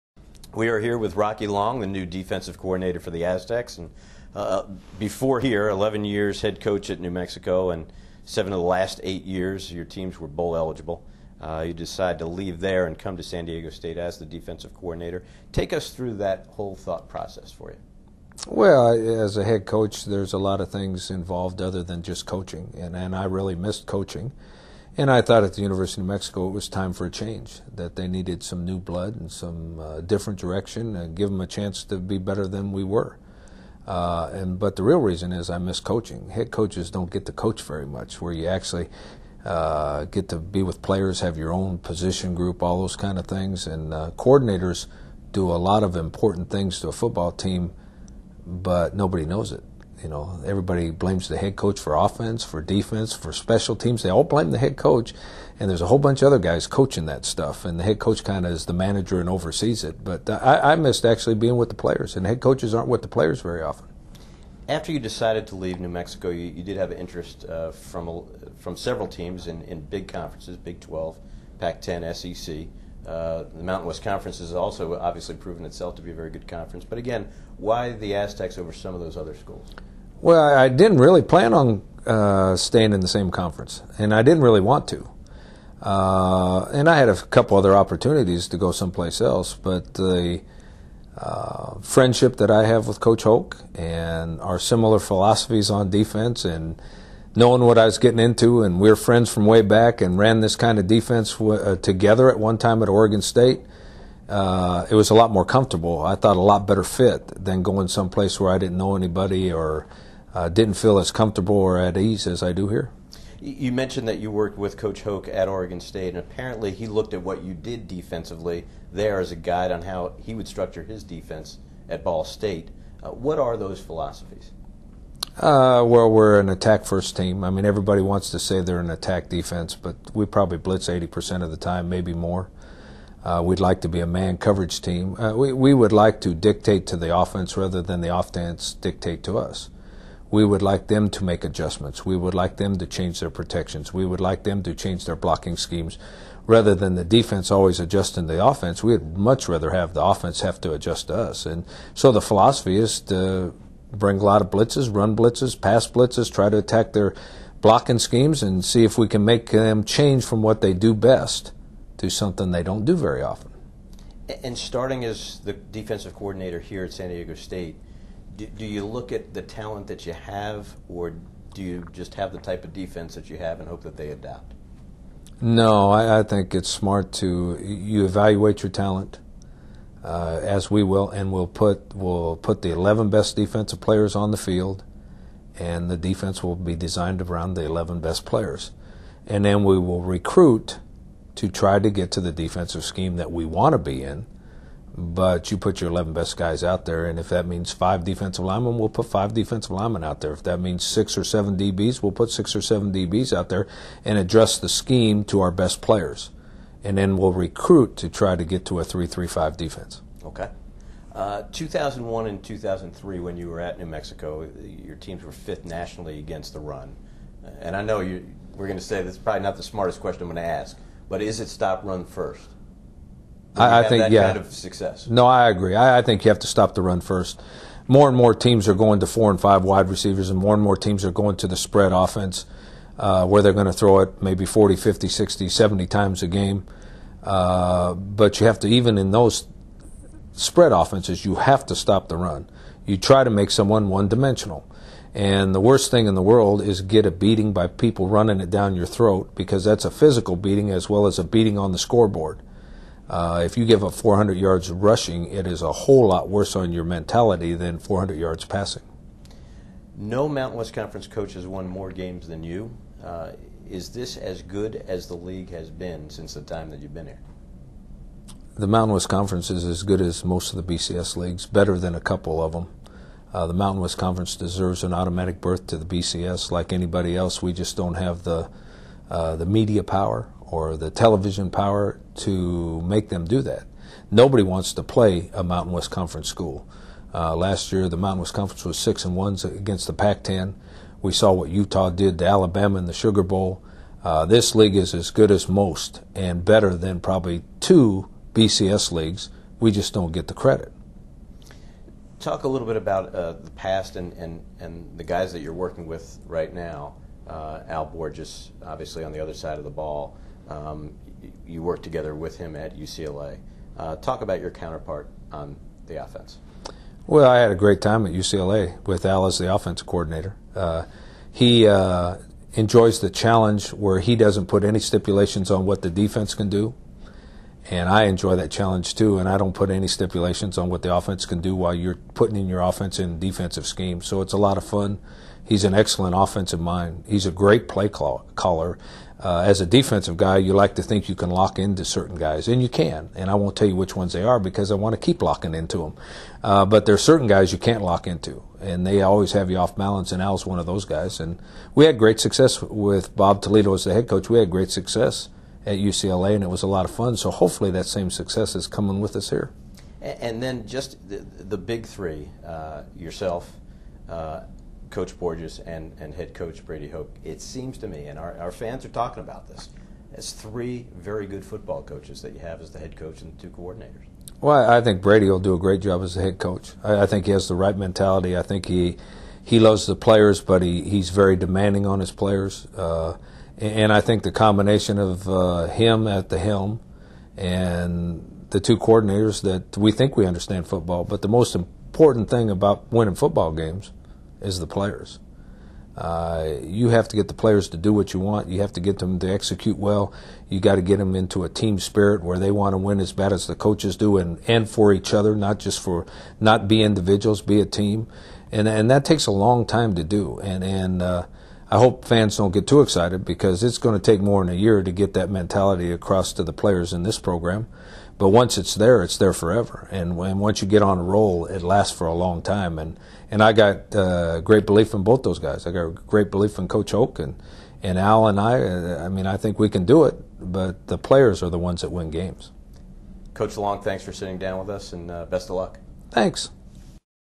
Rocky Long Interview
GoAztecs.com exclusive interview with new defensive coordinator Rocky Long.